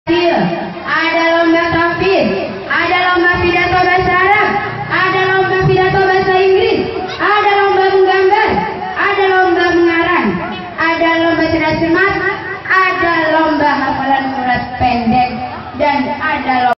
Open House of Ibadurrahman p.8
A students' performance of Ibadurrahman school in Ciruas Serang